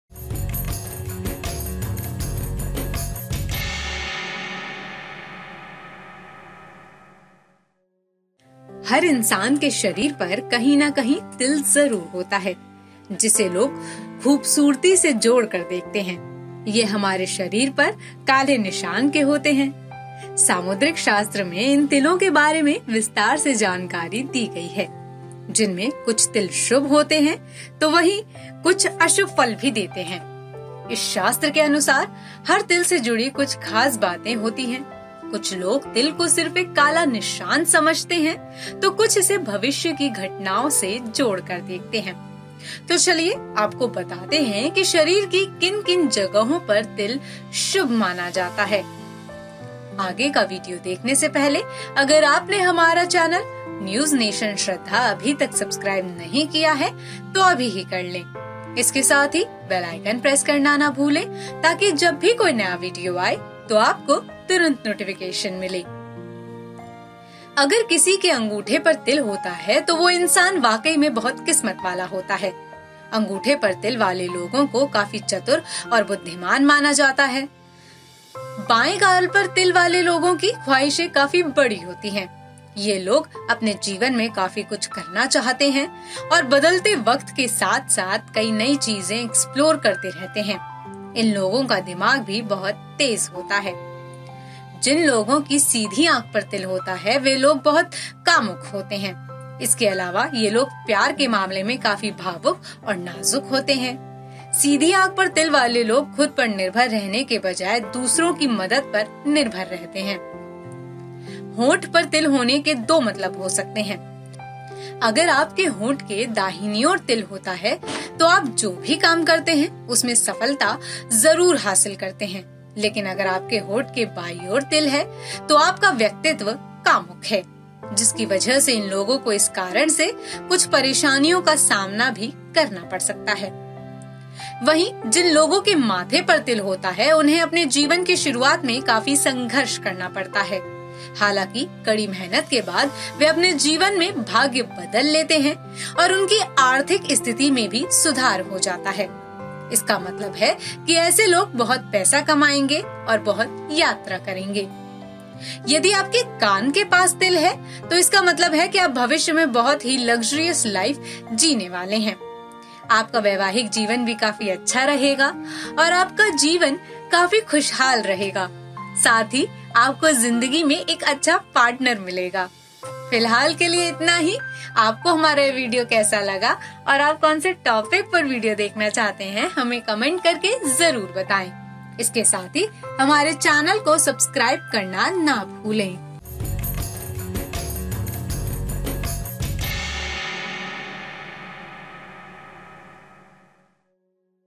शरीर के इन हिस्सों पर होता है तिल, होते हैं बुद्धिमान और काम में करते हैं सफलता हासिल
सामुद्रिक शास्त्र (samudrik shastra) में इन तिलों के बारे में विस्तार से जानकारी दी गई है. जिनमें कुछ तिल शुभ होते हैं. तो, वहीं कुछ अशुभ फल भी देते हैं. इस शास्त्र के अनुसार हर तिल (Mole) से जुड़ी कुछ खास बातें होती हैं. कुछ लोग तिल को सिर्फ एक काला निशान समझते हैं तो कुछ इसे भविष्य की घटनाओं से जोड़कर देखते हैं. तो, चलिए आपको बताते हैं कि शरीर की किन-किन जगहों पर तिल शुभ माना जाता है.